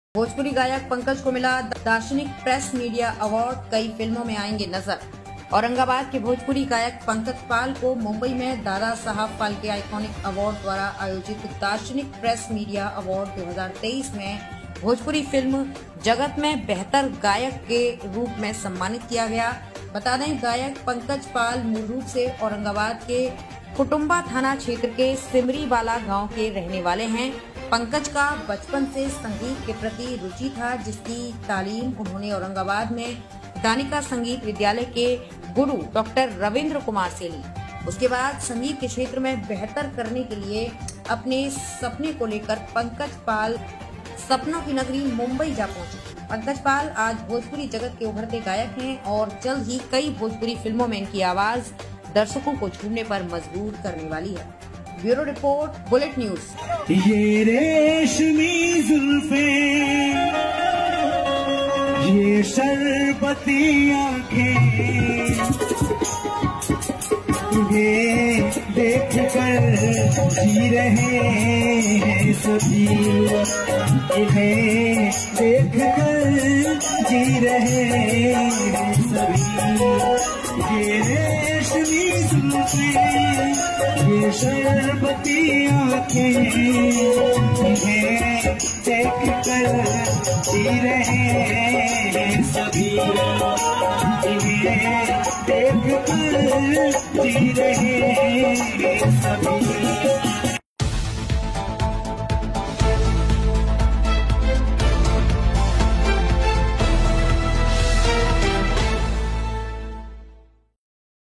बिहार के औरंगाबाद जिले के भोजपुरी गायक Pankaj Pal को दार्शनिक प्रेस मीडिया अवॉर्ड से सम्मानित किया
बिहार के औरंगाबाद जिले के भोजपुरी गायक Pankaj Pal  को दार्शनिक प्रेस मीडिया अवॉर्ड से सम्मानित किया, कई फिल्मों में आयेंगे नजर...!!
बिहार युवा गड़ेरिया मोर्चा परिवार की तरफ से बहुत बहुत बधाई हो आप ऐसे ही समाज का नाम रौशन करे...!!